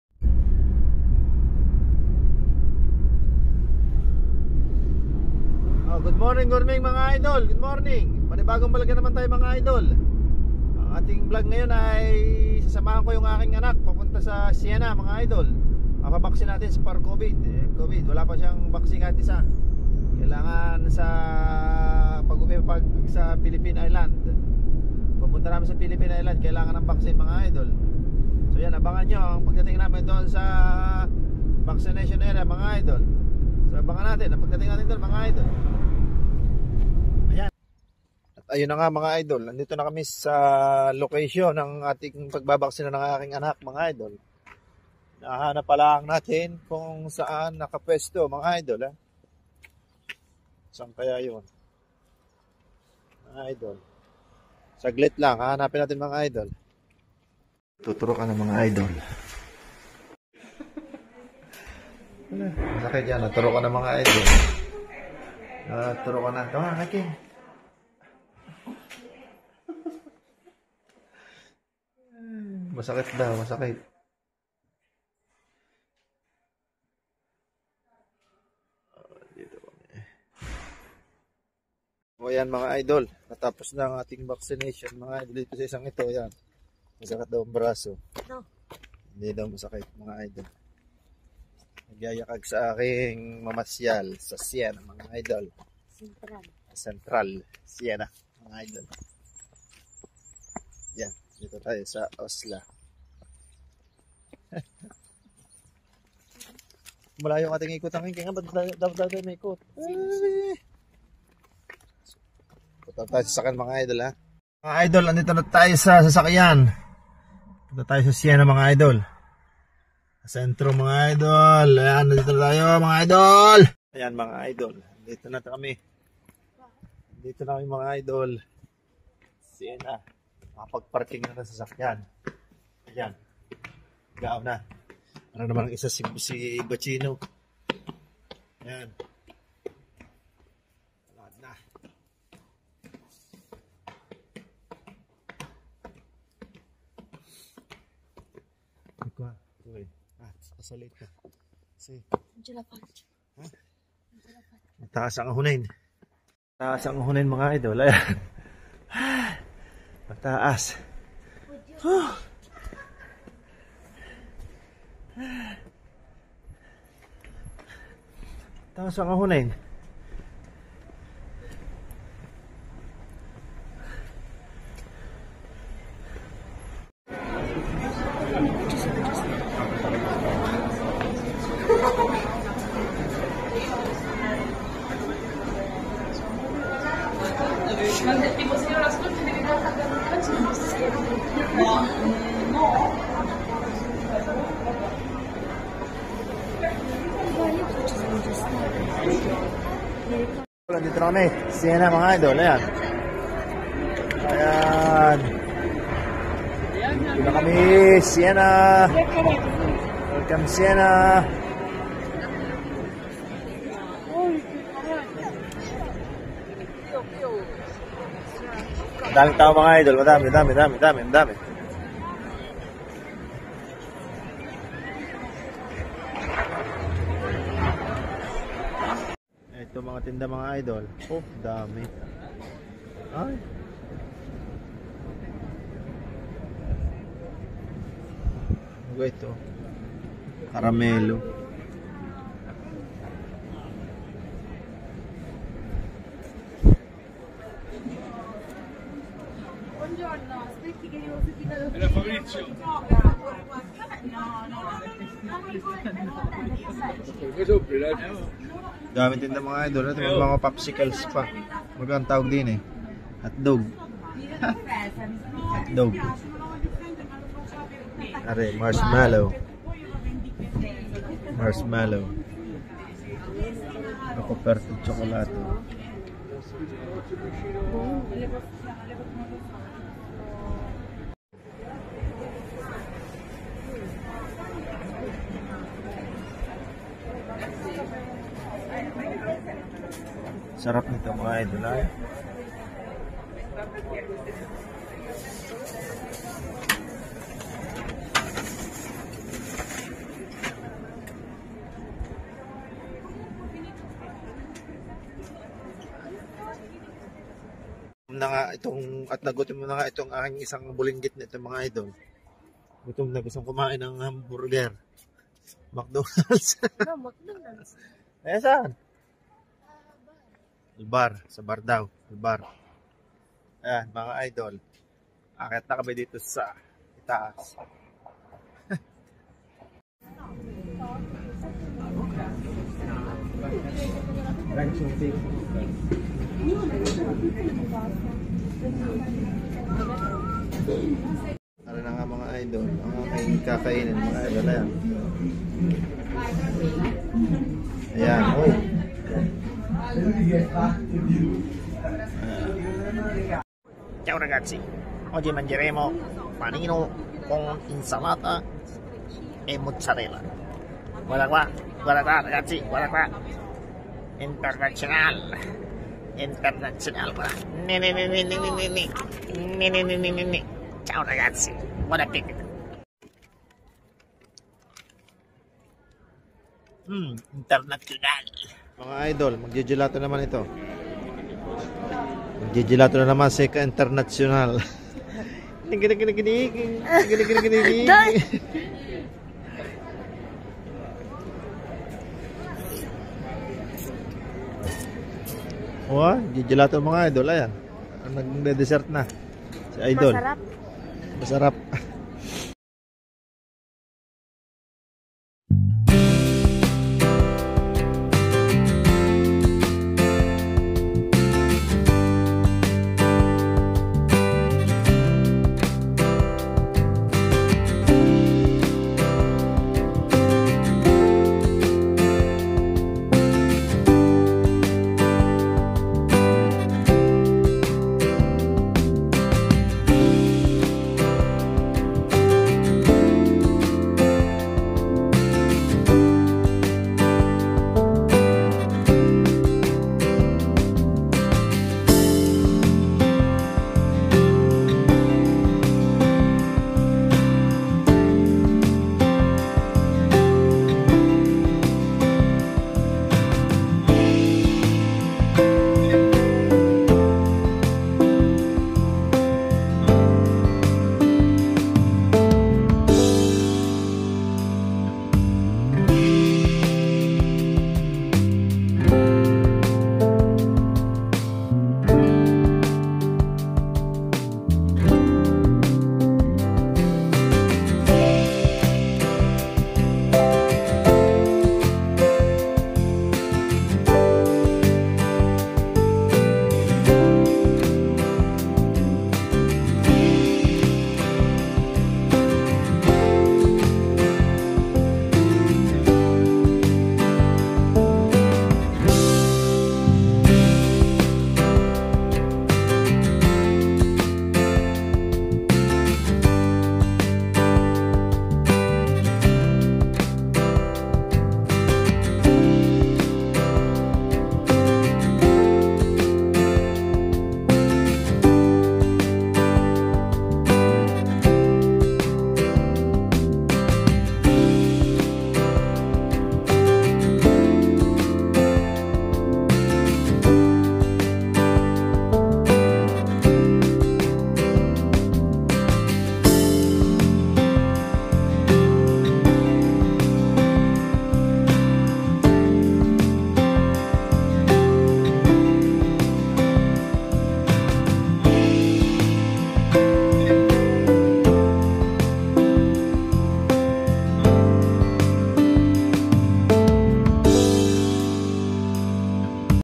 travelling to Siena,Italy
You should visit the medieval city of Siena for its stunning architecture, rich history, vibrant atmosphere, and fantastic local cuisine, not to mention its role as a gateway to explore Italy's famous wine regions.